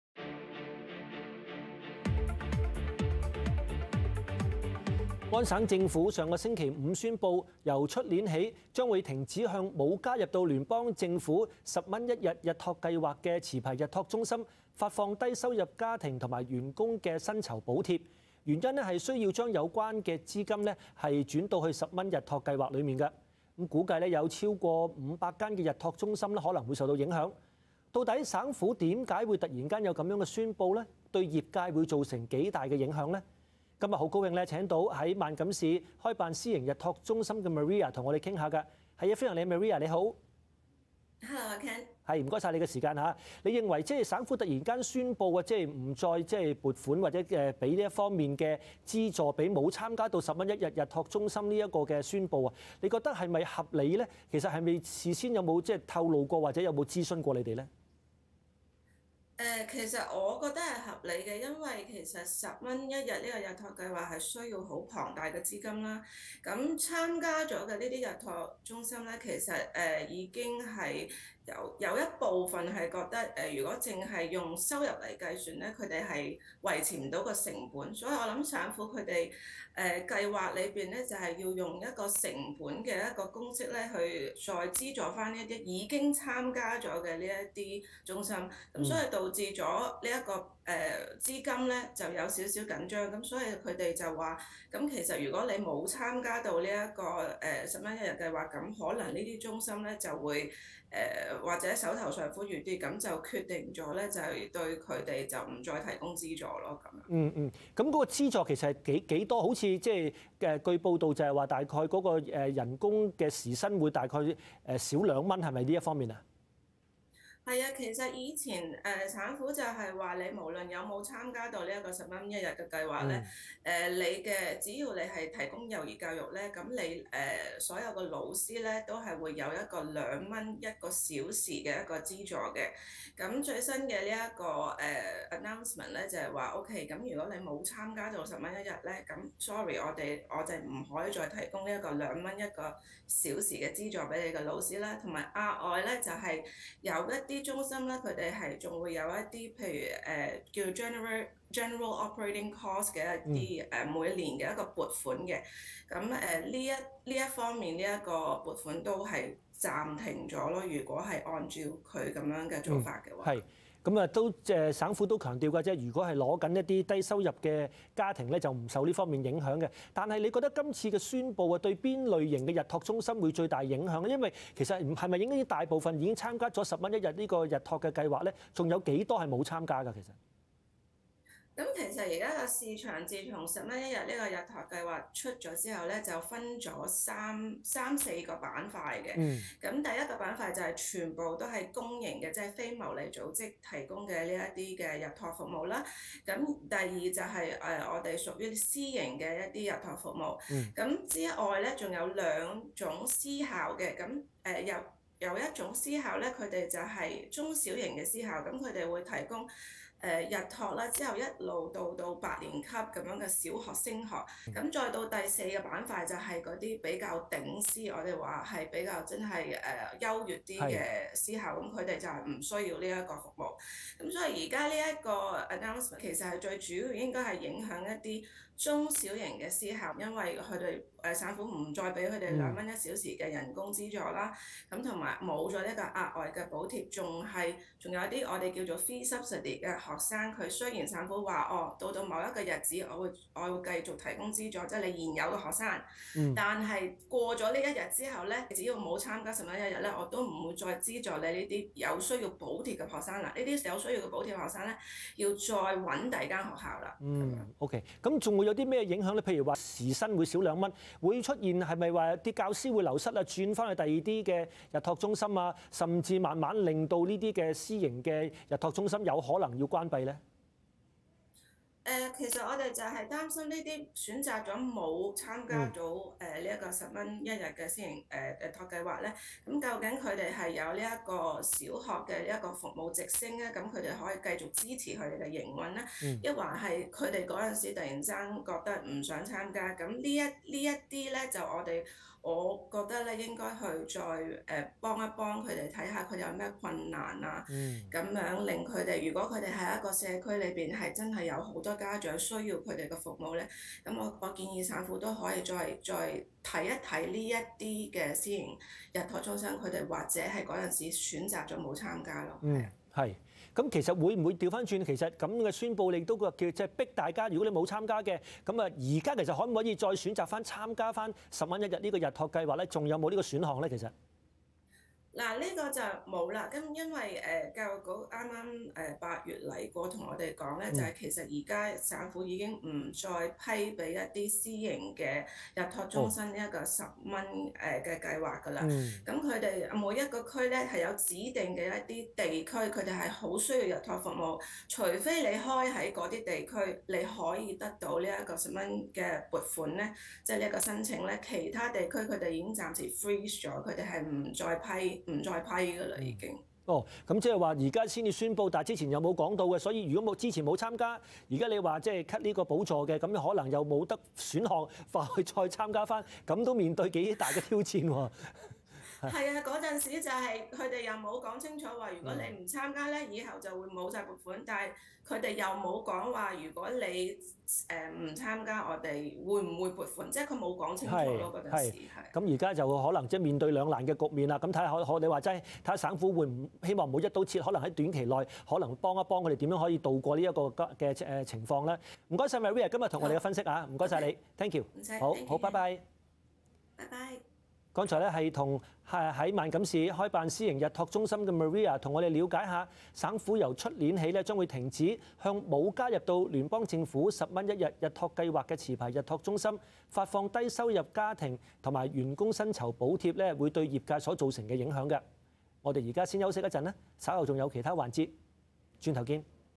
探討:省府削減部份日託中心的薪酬補貼 | OMNI News Cantonese
安省政府上星期五宣佈, 明年起, 將會停止向沒有加入聯邦政府10元一日日託計劃的持牌日託中心，發放低收入家庭和員工薪酬補貼，原因是需要將有關撥款轉到10元日託計劃，估計有超過5百間日託中心受影響. 到底省府為何突然有這樣的宣佈? 對業界會造成多大的影響? 專訪於萬錦市開辦私營日托中心的Maria。